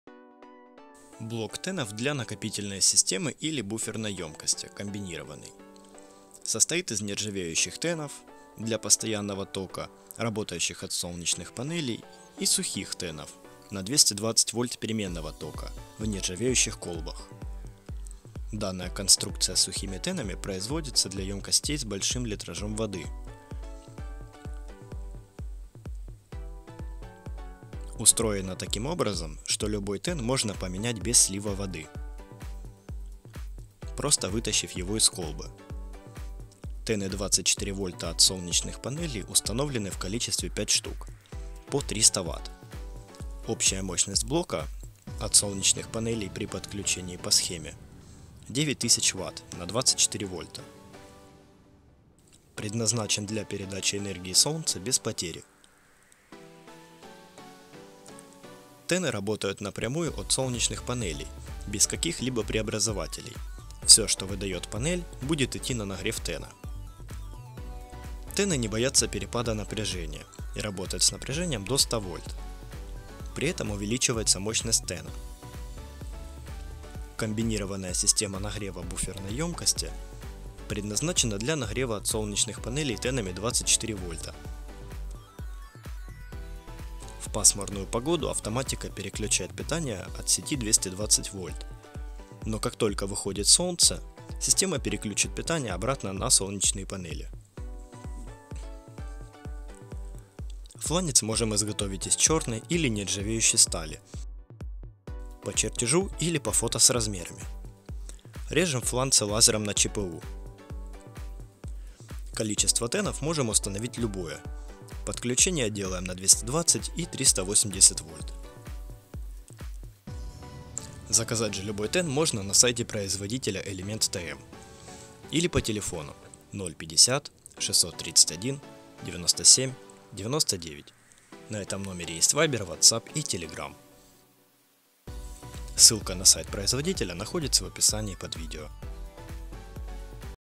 Отопление постоянным током от солнечных панелей 100% без потерь.
Блок тэнов для накопительной системы или буферной ёмкости.

Изготовлен по заказу для Европы.
70% частных домов в Европе нагревают бойлер с помошью твердотопливных котлов, используя горючее твёрдое или жидкое топливо.
Даже летом можно заметить дым из трубы.
Всё это лишние, вредные выбросы в атмосферу. С чем так настойчиво борется Европа.
 Комбинированный блок состоит из нержавеющих тэнов для постоянного тока работающих от солнечных панелей и сухих тэнов 220 в переменного тока в нержавеющих колбах. Данная конструкция с сухими тэнами производится для емкостей с большим литражём воды.
Устроена таким образом, что любой тэн можно поменять без слива воды, просто вытащив его из колбы. Тэны 24 вольта от солнечных панелей, установленны в количестве 5 штук по 300 вт, общая мощность блока от солнечных панелей при подключении по схеме (фото) 9000 вт на 24 в, предназначен для передачи энергии солнца без потери.

Тэын работают на прямую от солнечных панелей, без каких либо преобразователей. Всё, что выдаёт панель, будет идти на нагрев тэна.

Тэны не боятся перепада напряжения и работают с напряжением до 100 вольт. При этом увеличивается мощность тэна.

Комбинированная система нагрева буферной ёмкости предназначена для нагрева от солнечных панелей тэнами на 24в, 48в или 64в.
 В пасмурную погоду, автоматика переключает питание от сети 220 в, но, как только выходит солнце, система переключит питание обратно от солнечных панелей. Для этой системы на фланце установлена трубка под термостат.                                                     Фланец можем изготовить из чёрной и нержавеющей стали. По чертежу, по фото с размерами. Режем фланцы лазером на ЧПУ. Количество тэнов можем установить любое, подключение делаем на 220 и 380 в